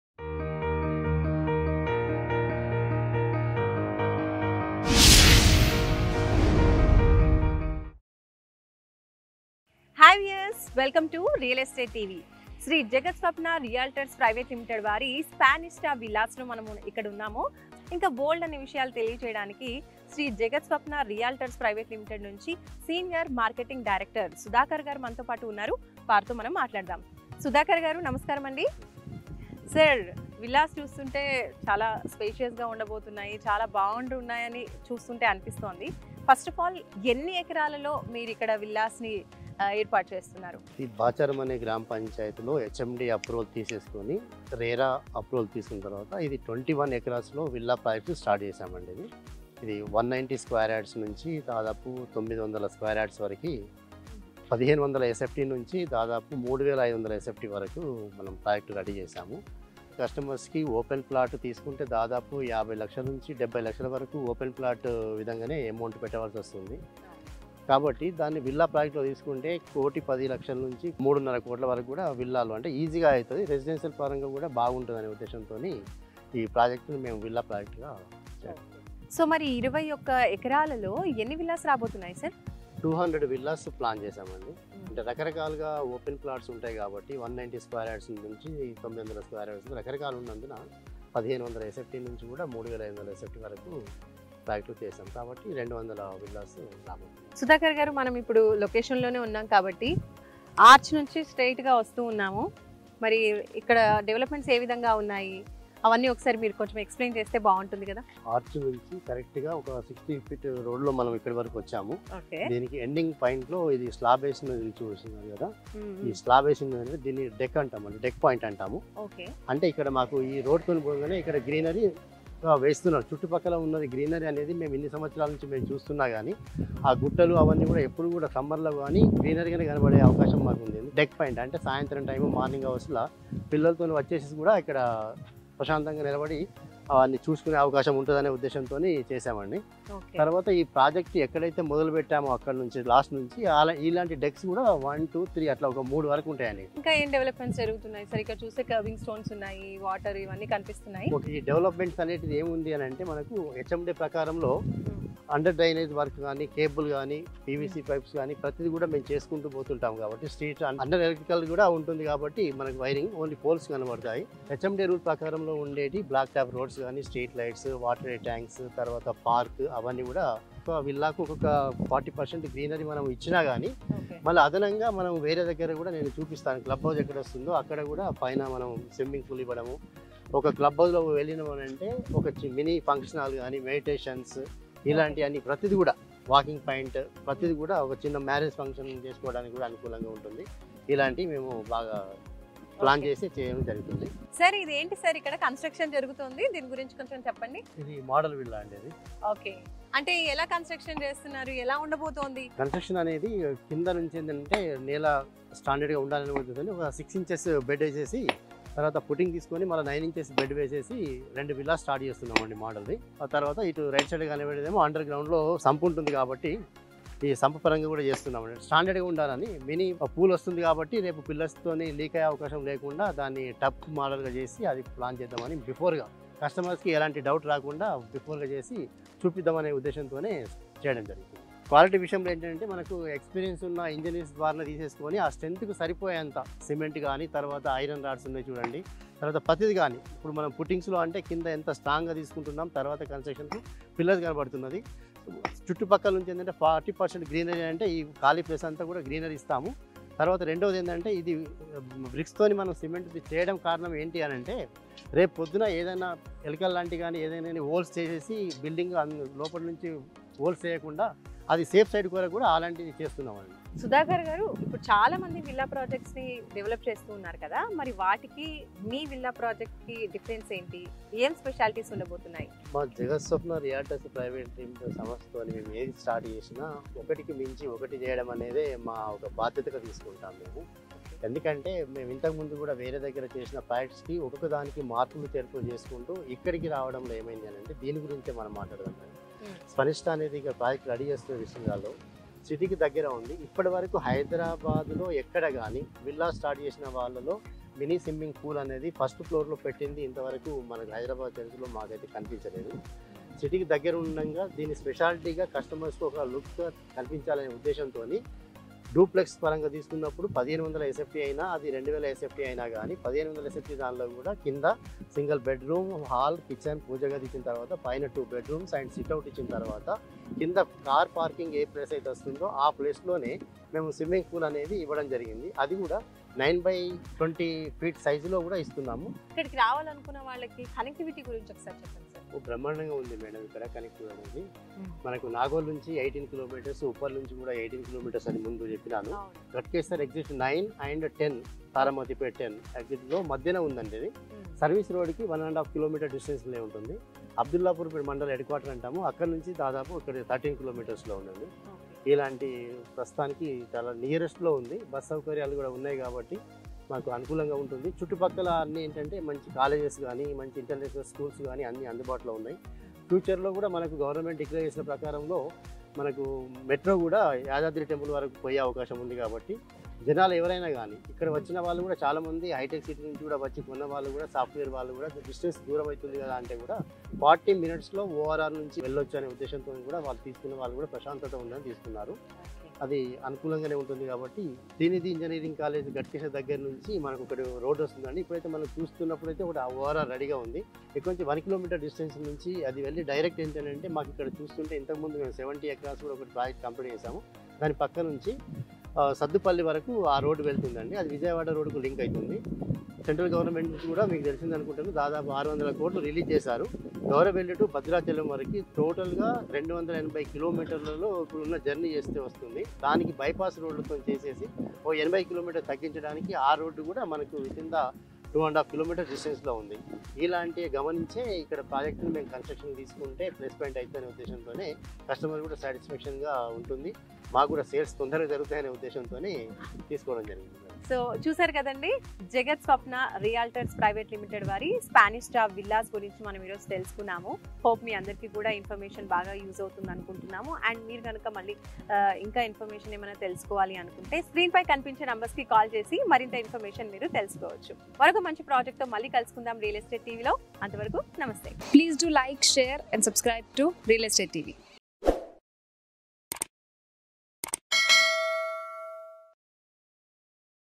SPANESTA VILLAS | 200 Luxury Villas at Taramatipet ORR Exit | Villas near ORR సర్వీస్ రోడ్ కి దగ్గర
SPANESTA VILLAS || 200 Luxury Villas at Taramatipet ORR Exit | ORR సర్వీస్ రోడ్ కు 650 మీటర్ల దూరంలో

Jagathswapna realtors Pvt ltd...presents spanesta villas at bacharam
➡️Hmda and rera approved project
Gated community features 100%vastu
➡️25acres...200 villas
➡️190 sqy to 900 sqy
1500 sft to 3000 sft
➡️Luxurious villas with unique concept of individual swimming pools for every villa...
➡️In Entire layout only 60% for construction and 40% for greenery
➡️The project is facing 100ft radial road leading to Orr upto Chowdhury guda
➡️The project also has 150ft master plan road on the other side
➡️The amenities offering are
30&40 ft roads
Underground drainage electricity water
Grand entrance arch24/7 security
Entry only through my gate app
Parks
street lights
 harvesting pits..
Avenue plantation.
Buddha vanam
cc camera
club house with food court meditation hall sport segment amphitheatre..walking and jogging track...
➡️The brand used for construction are all A1 brands...for that read specifications throughly
➡️Price starts from 1cr and last upto 4cr

Bank loan avbl

➡️Location highlights
half km to Orr
Very close to 3 orrs
Ghatkesar..taramatipet..pedda amberpet
5km to Pocharam Infosys campus
3mints drive to uppal metro
Surrounded by schools colleges hositals shopping malls
Close by Aims yadadri temple many more
Lastly the apartments rates in pocharam area costs around...
➡️6500 to 7000 per sft
Whereas here in the same cost customer can possess a villa in a community with gated community features